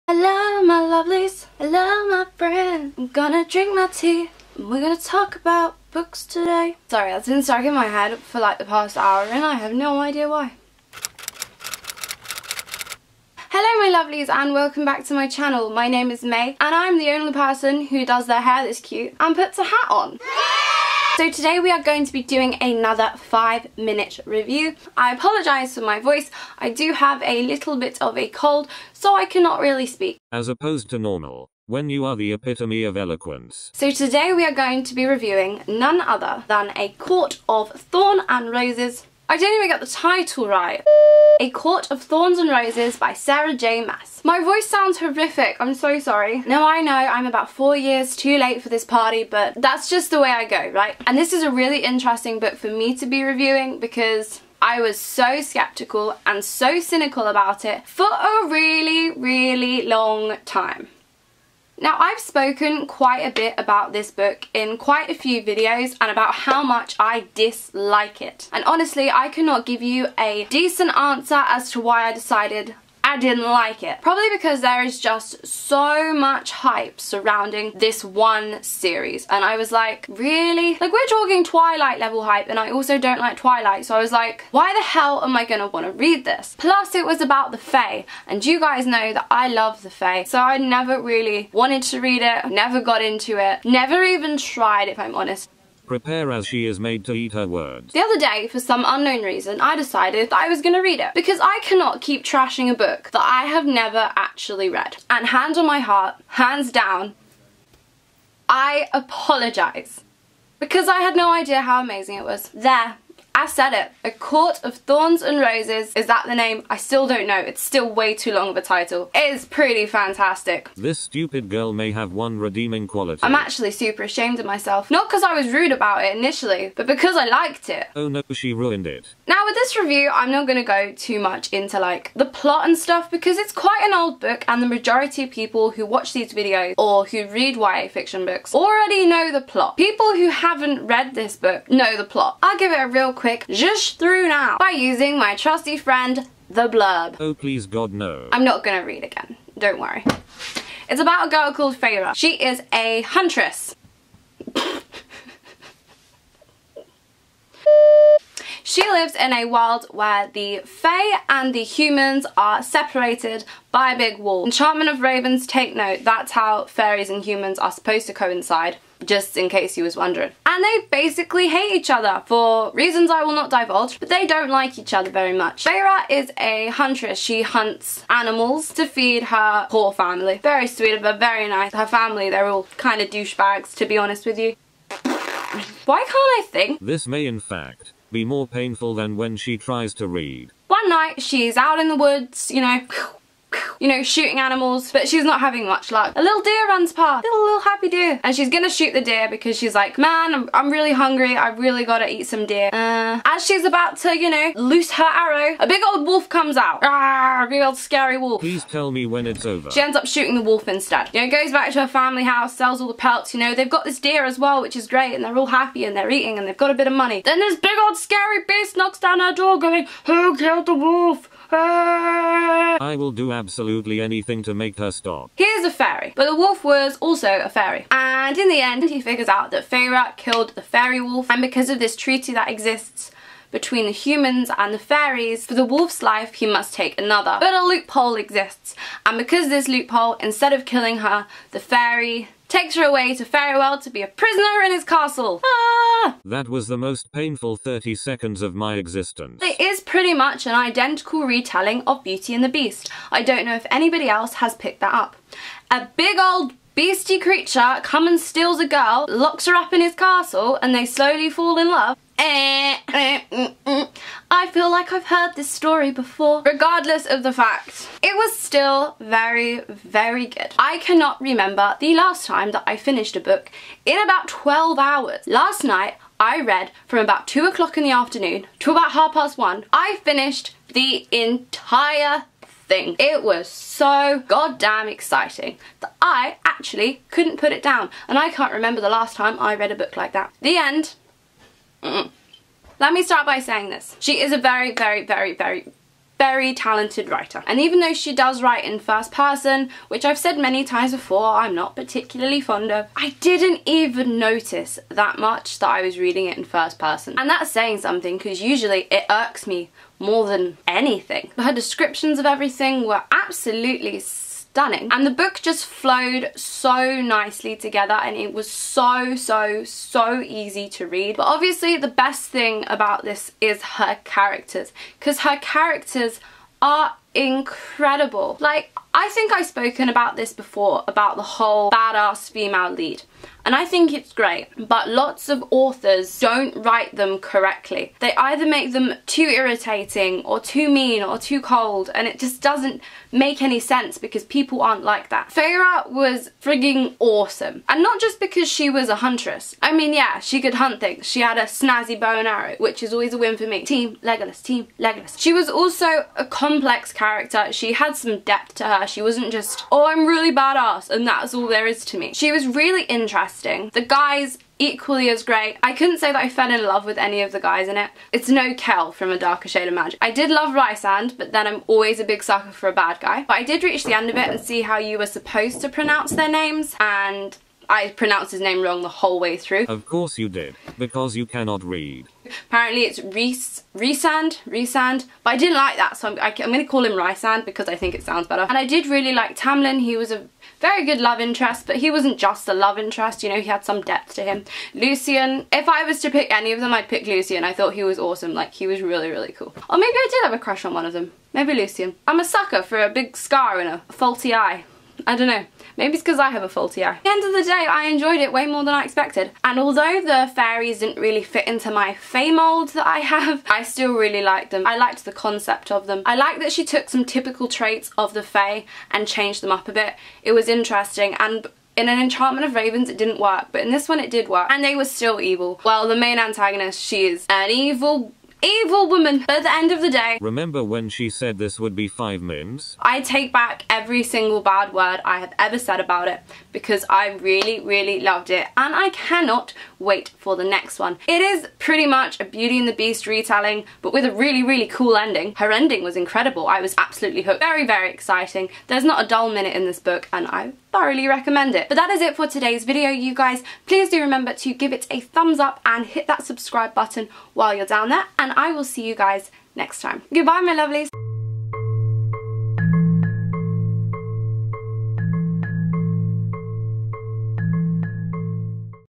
A COURT OF THORNS AND ROSES - 5MIN REVIEWS / Mae in Wanderland
I'm back again with another episode of 5min reviews, this time on A Court of Thorns and Roses by Sarah J.Mass. This is not a book I ever thought I'd read let alone review!

In the vid I give a quick recap of the first couple of chapters, but no major spoilers!

Mae
x